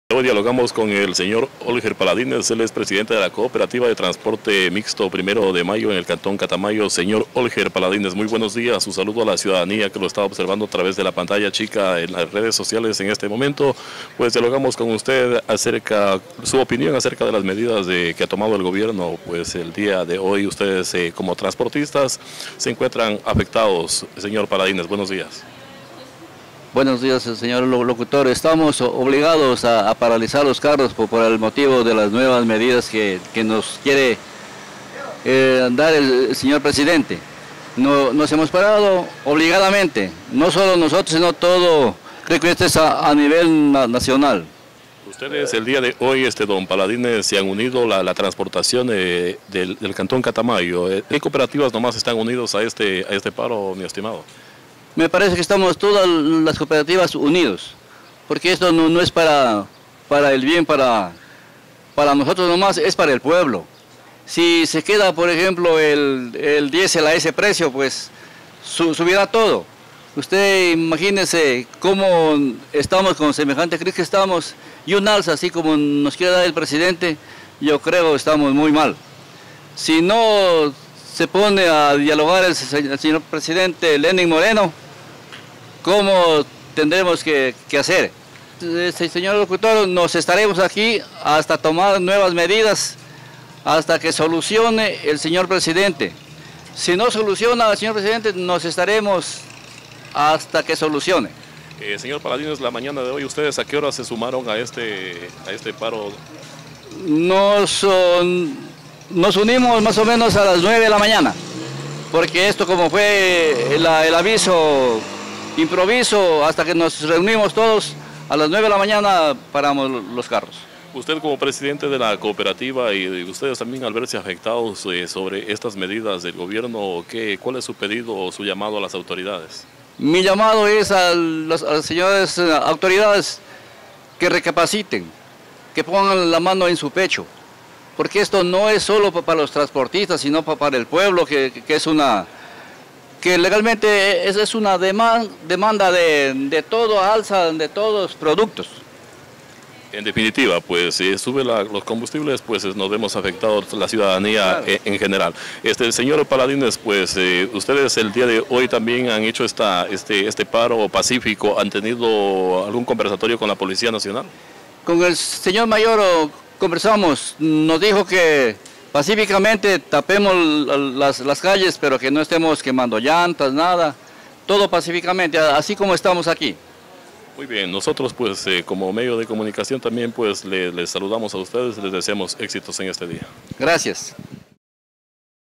Olger Paladines -Pdte. Coop de Transporte mixta “1ro. de mayo” Paro Nacional 2019
El trasporte del cantón Catamayo se une al Paro Nacional
Olger Paladines -Pdte. Coop de Transporte mixta “1ro. de mayo” da a conocer su punto de vista sobre la medida económica, dadas a conocer por el gobierno nacional Lenin Moreno.